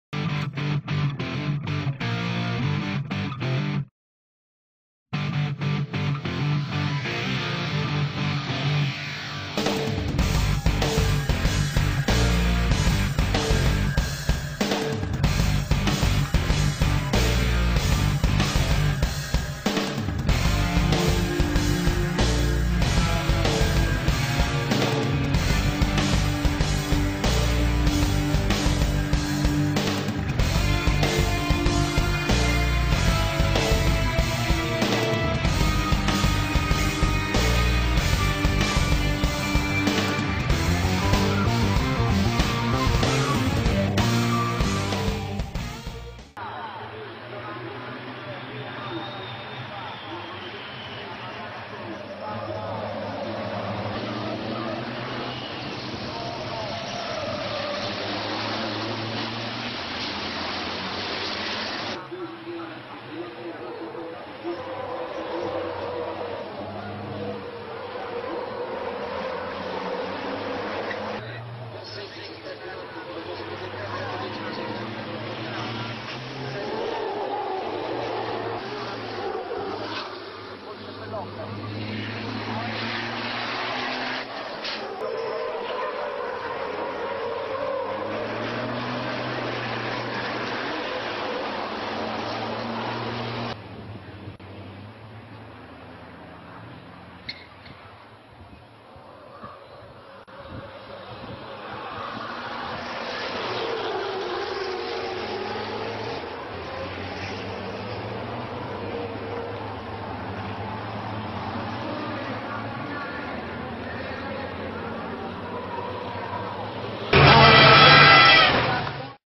Caterpillar race truck
A tribute to this fantastic race truck. It includes races and photos